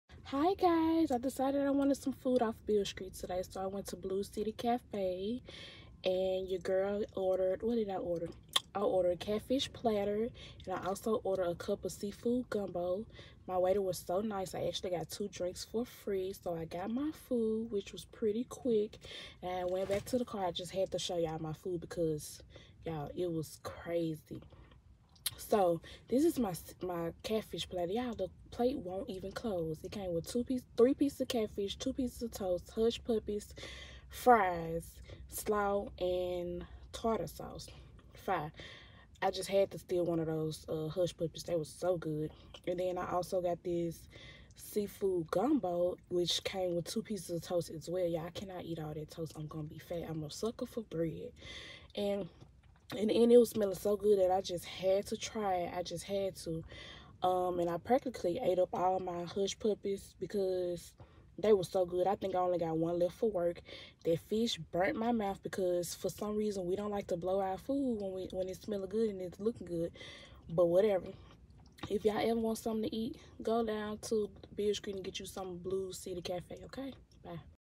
BLUES CITY CAFÉ | BEST OF BEALE STREET | FOOD LOVER
Today’s food of the day is Beale Street’s own Blues City Café.